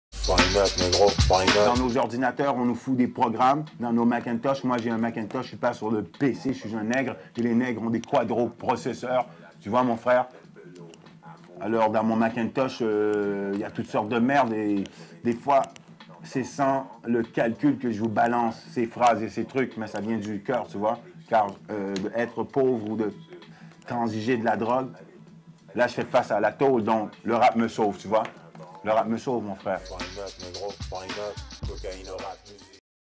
Interview - Le Roi Heenok et l’Informatique (200?)
Le Roi nous parle de ces préférences d'ordinateurs.